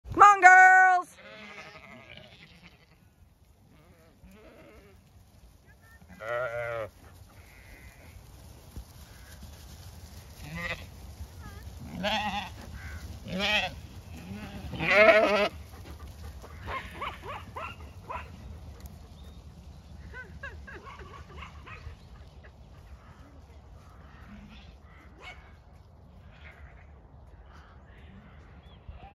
The world’s smallest sheep dog makes sure straggler sheep keep up
Inca loves her job of herding our flock of Zwartbles sheep.